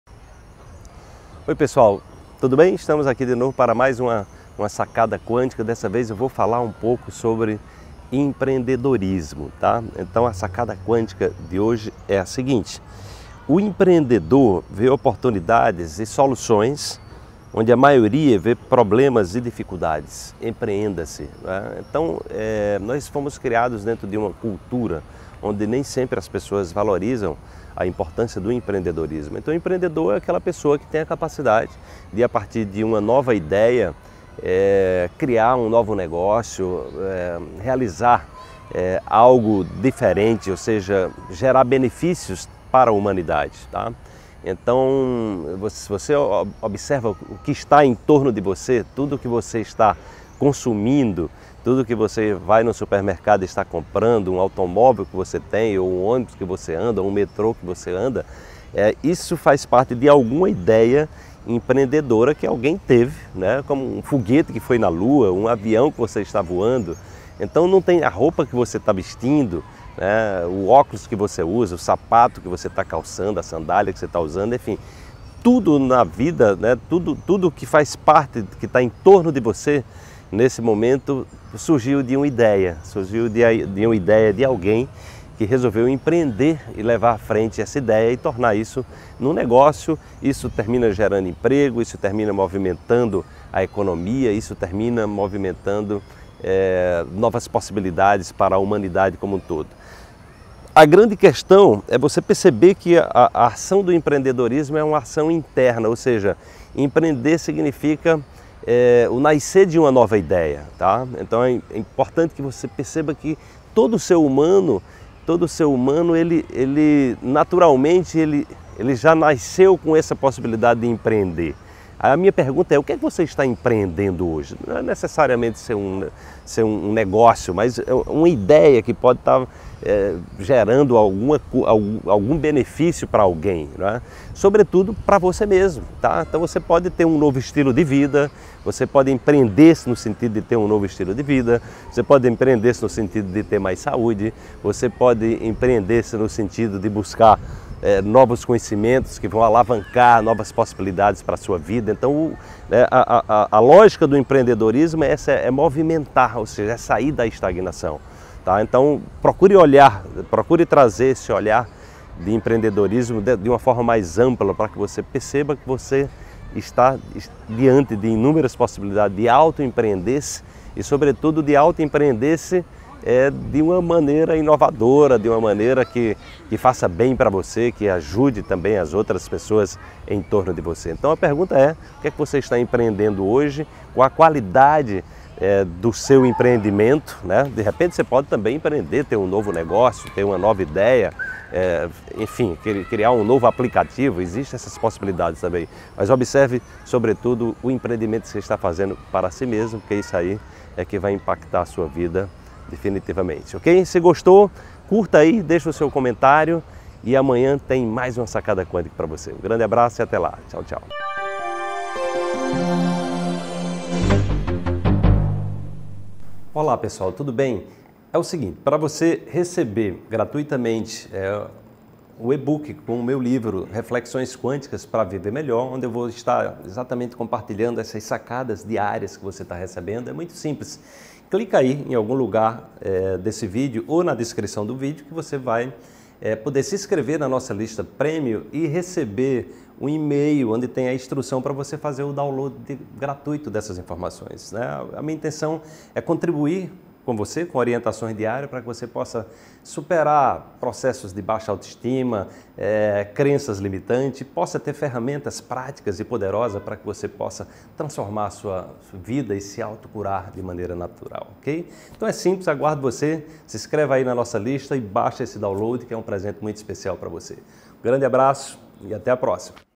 Coaching Quântico 093: Desperte o Empreendedor em Você! | Wallace Liimaa
✭✭✭✭✭ Nesse vídeo abordo a importância de exercer a criatividade e empreender.  Assista já!